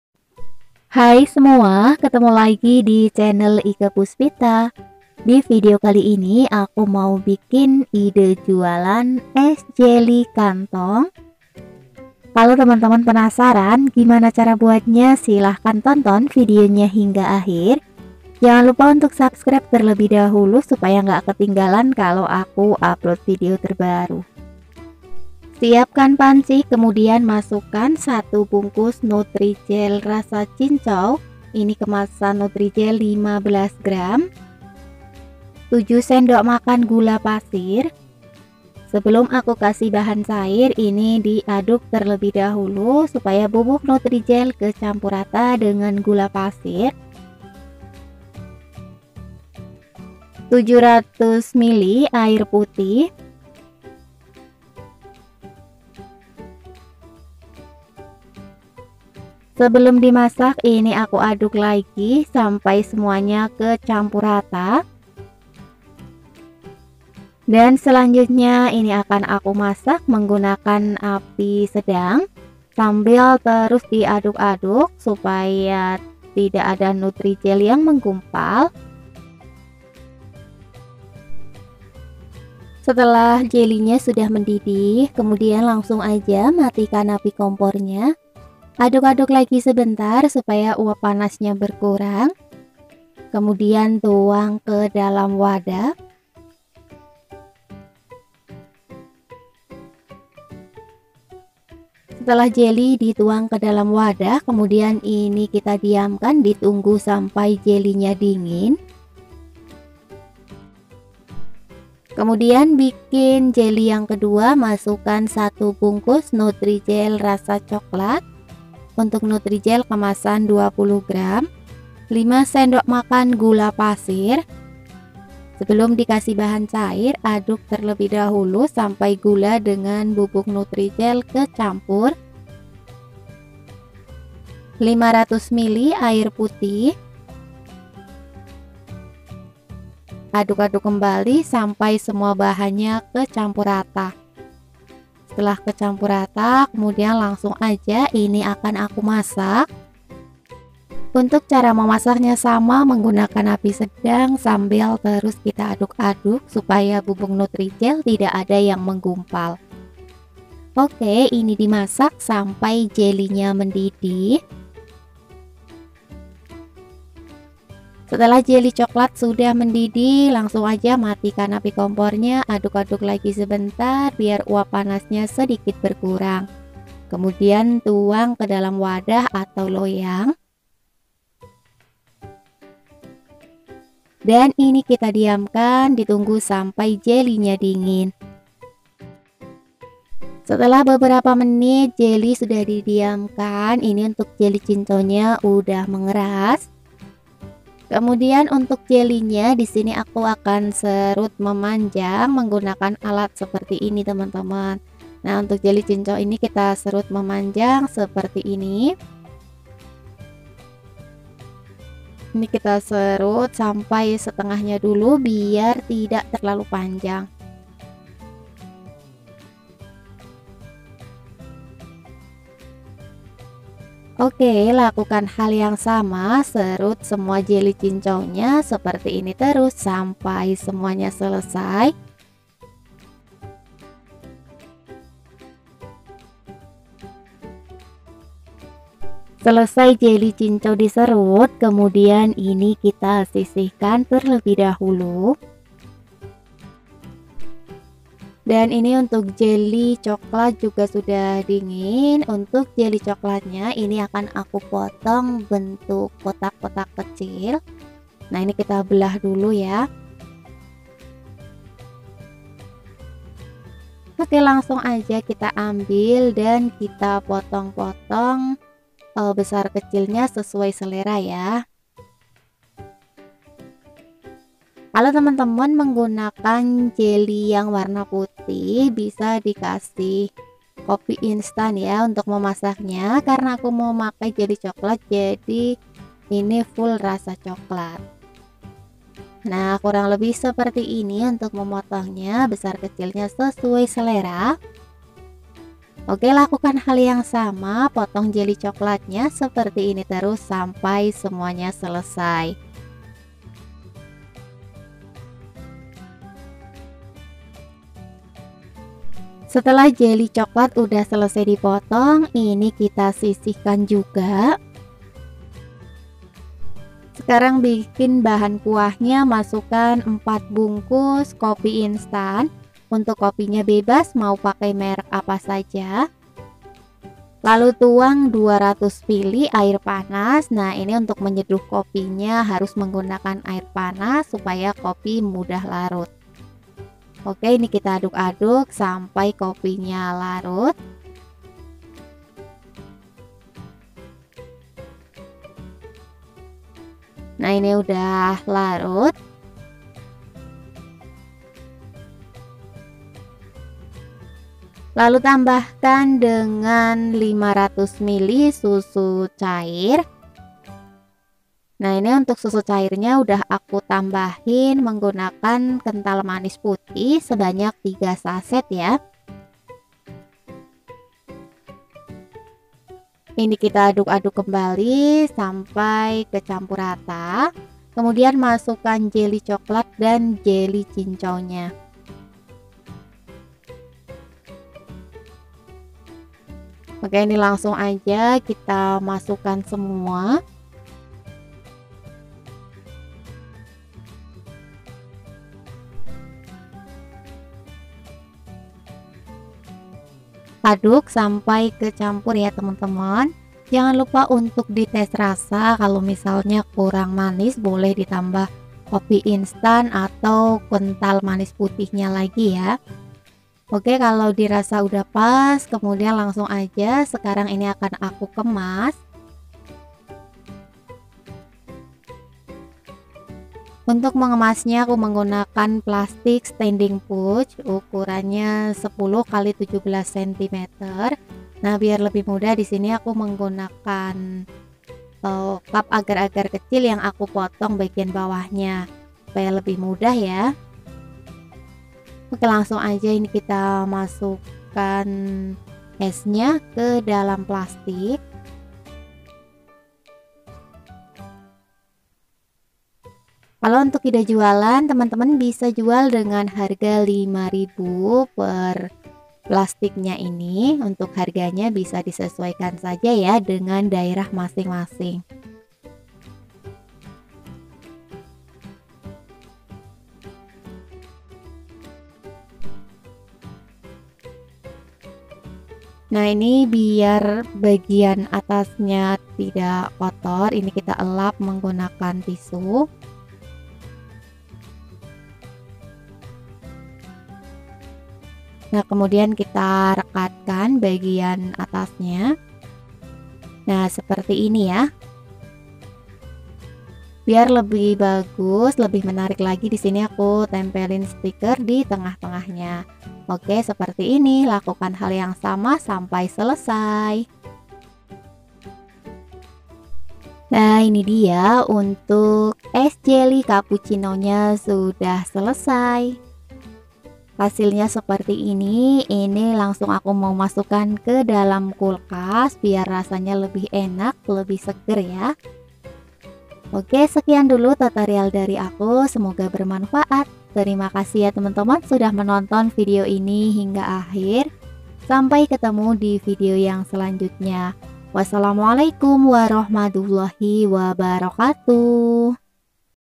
KUASAI RESEP INI! BERSIAPLAH PUNYA CAFE SENDIRI | Es Kantong Cappucino Jelly Ide Jualan Minuman Unik
Assalamualaikum hai saya Ike Puspita. Di video kali ini saya share resep membuat es kopi susu jelly Ekonomis yang enak dan segar ala cafe. Cocok untuk ide jualan titip warung, maupun ide jualan online karena modalnya sangat sedikit dan potensi untungnya lumayan.

Tau sendiri lah ya jualan minuman kekinian marginnya berapa. Saya jelaskan lengkap, detail dan rinci takarannya, jadi pastikan tonton dan simak videonya hingga akhir yah

Berikut bahan-bahannya:

Jelly cincau:
1 bungkus nutrijell cincau
700 ml air putih
7 sdm gula pasir

Jelly coklat:
1 bungkus nutrijell coklat
5 sdm gula pasir
500 ml air putih

Bahan kuah:
4 bungkus kopi instan
3 sacset skm
200 ml air panas
500 ml susu full cream

Silakan LIKE, KOMEN, dan SHARE video ini jika dirasa bermanfaat untuk temen/ sodara kalian